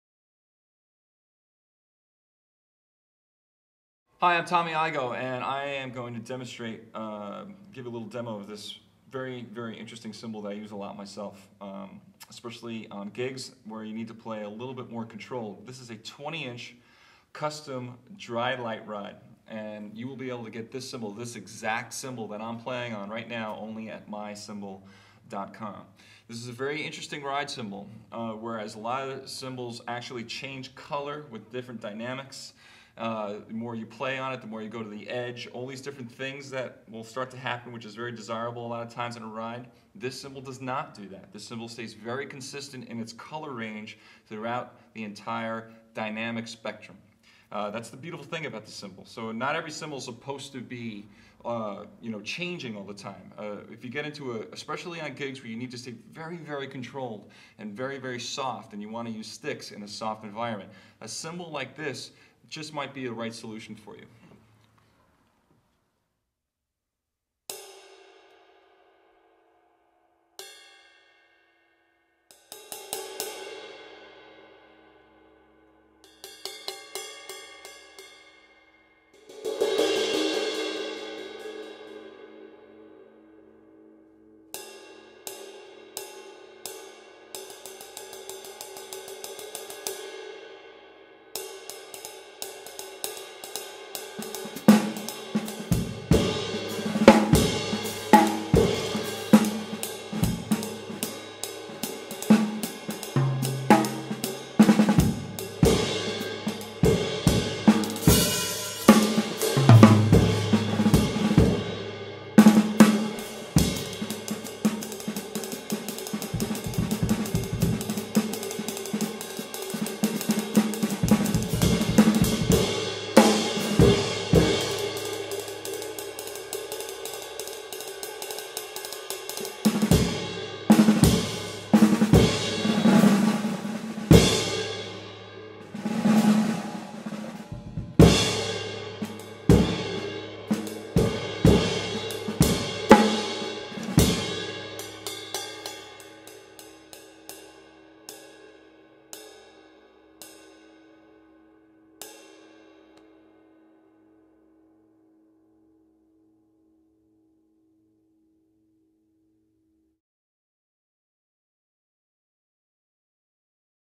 Zildjian 20" K Custom Dry Light Ride Cymbal - Played by Tommy Igoe (K0966-1072210A)
Zildjian 20" K Custom Dry Light Ride Cymbal at Memphis Drum Shop

Other gear used in this video:
• Cymbals (From drummers L to R):  Zildjian 14" K Custom Dark Hi Hat Cymbals, Zildjian 17" K Dark Thin Crash Cymbal, Zildjian 20" Oriental Crash of Doom Cymbal
• Drum kit:  DW Collector's Series Maple
• Snare:  DW 13x5.5 Collector's Series Aluminum Snare Drum
• Sticks:  Vic Firth Signature Series - Tommy Igoe Drumsticks